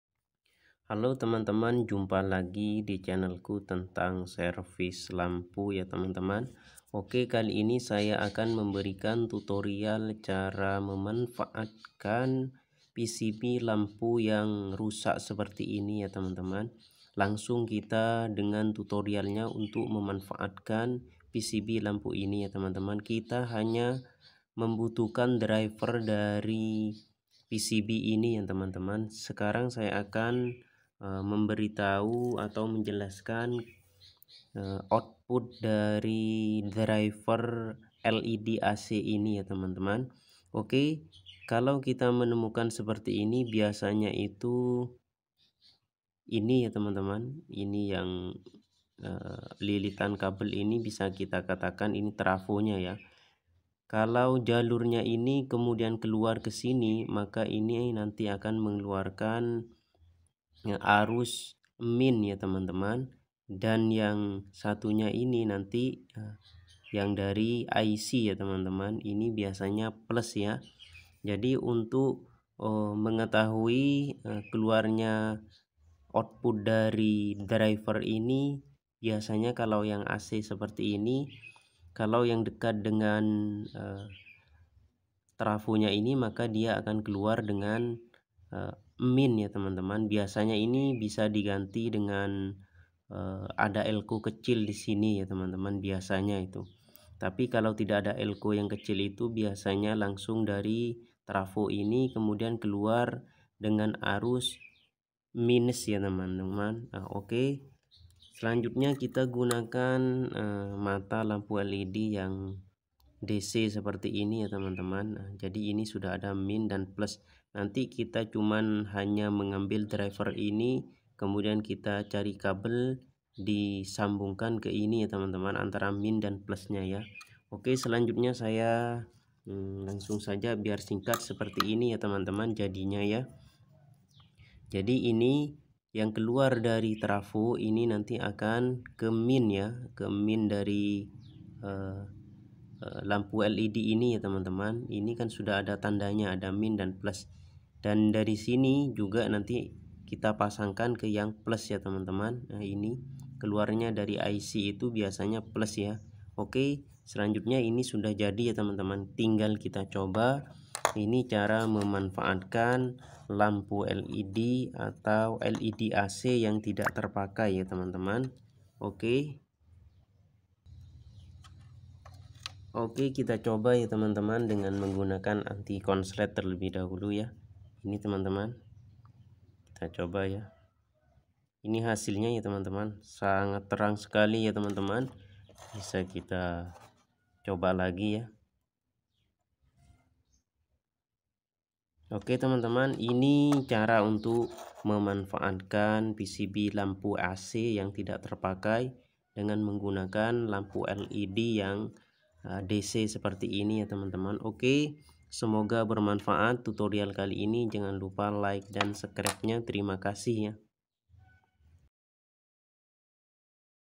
MEMANFAATKAN PCB LED AC YANG SUDAH TERBUANG
tutorial kali ini memanfaatkan PCB led AC yang rusak dan tidak terpakai

Hallo teman- teman jika membutuhkan peralatan servis lampu led,LHe, dan peralatan electronic lainnya yang murah dan berkualitas kunjungi link di bawah ini